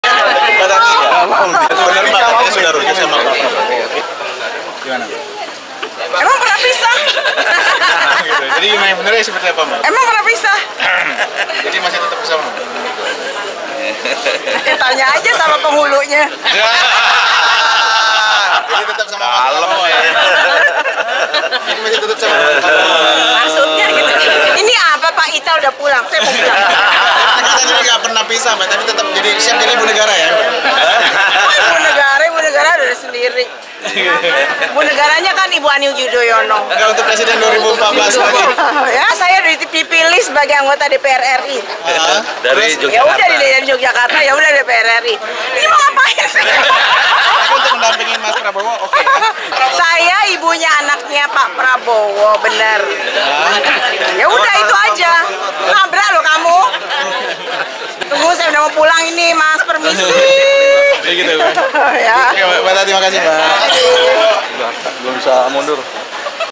Titiek Soeharto Bantah Pisah Dengan Prabowo
Politisi Golkar, Titiek Soeharto membantah jika dirinya telah berpisah dengan suaminya, Prabowo Subianto yang kini menjadi Capres pada Pilpres 2014